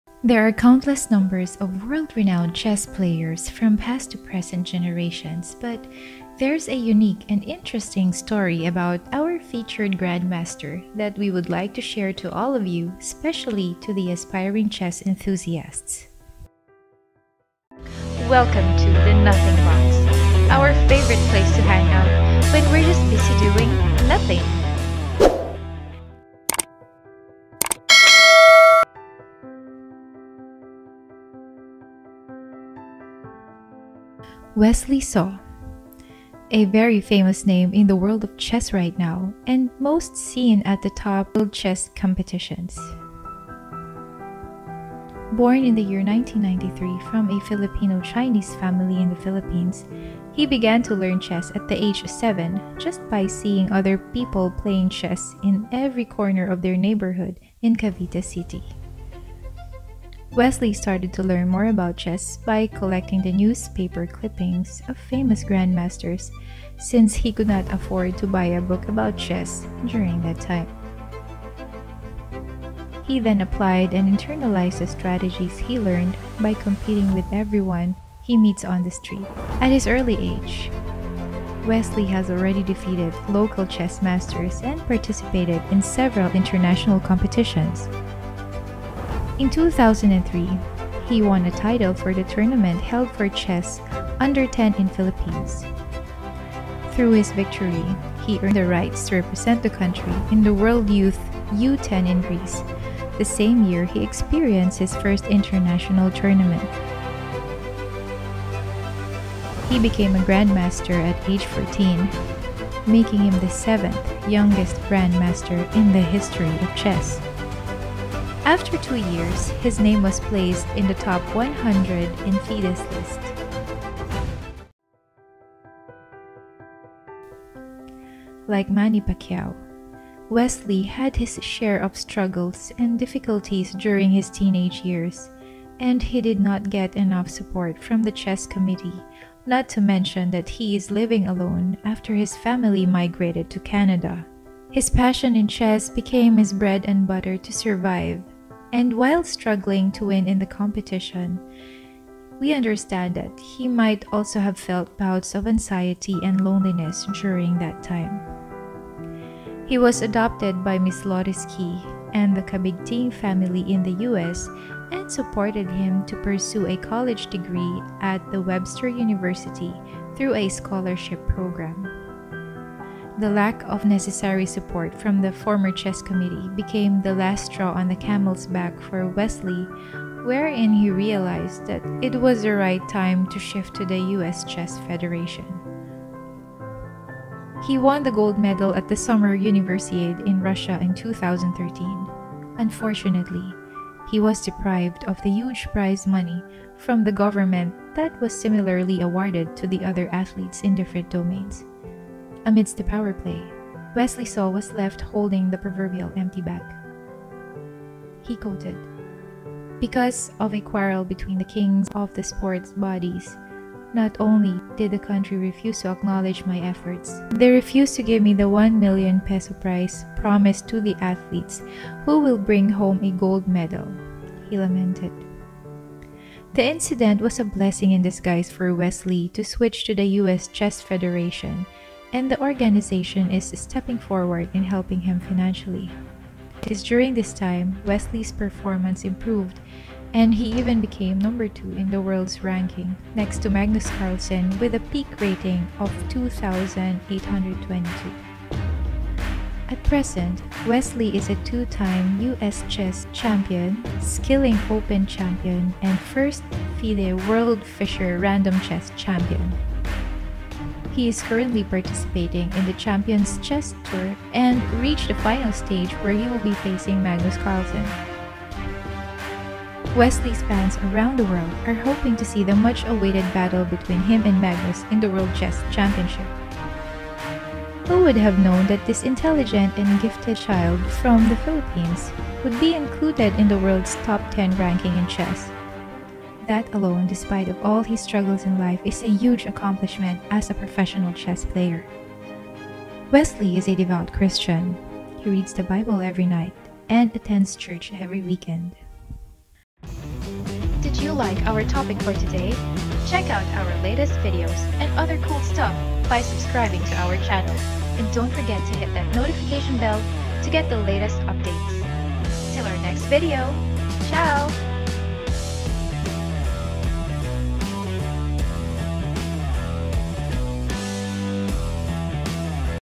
THE MAKING OF A CHAMPION - GM Wesley So
A short video about the how GM Wesley So, paved his way to establish his name and career in the field of CHESS all over the world.